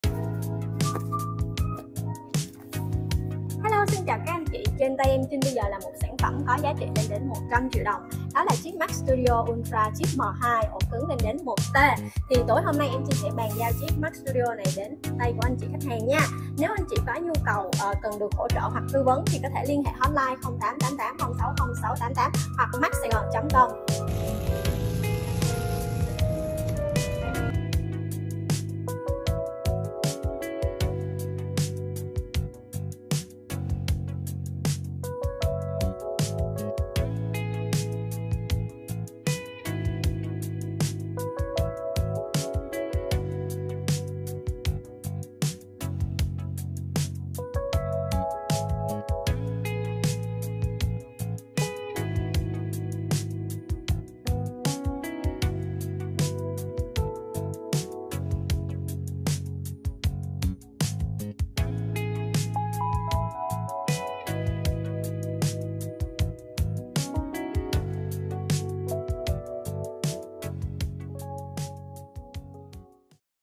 Trung thu với đèn lồng Apple gần 100 triệu?
Chuyện vui trung thu năm nay, em khoe với các Anh Chị đèn lồng kéo quân gần 100 triệu thật đẹp của Apple, em nghe lén anh khách nói đây là phần thưởng tốt nghiệp thủ khoa, em chúc ảnh sự nghiệp luôn sáng ngời như trăng trung thu cùng lồng đèn rực rỡ này.